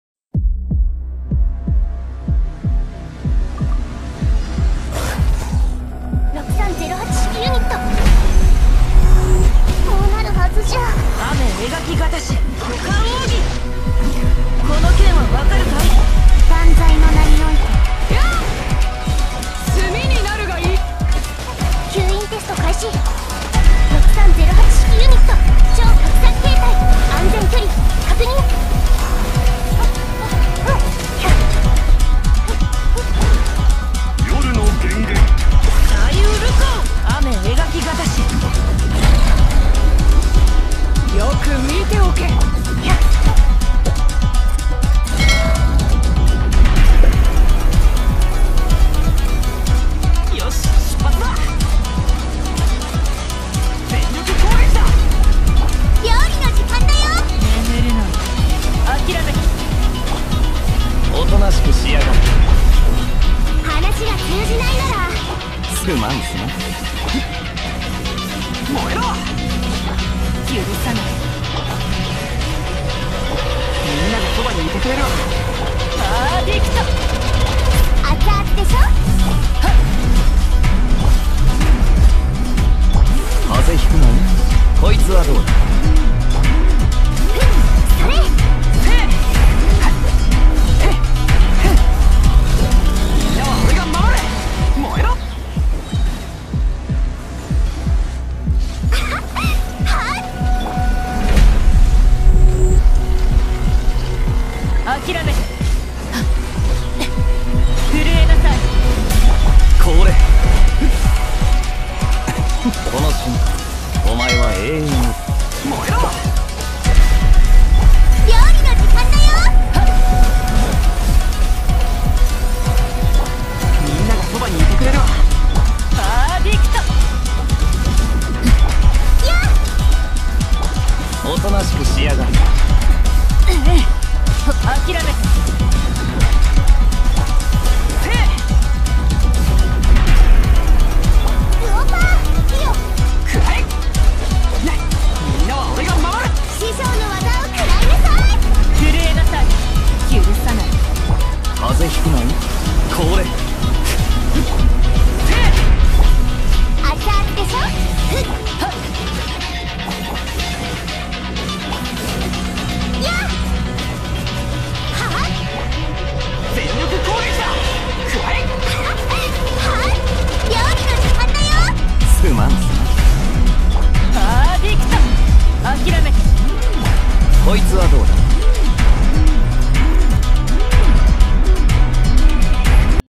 Which 4 Star Characters Worth Building
If you are aiming for a spiral abyss full star
then these character are the key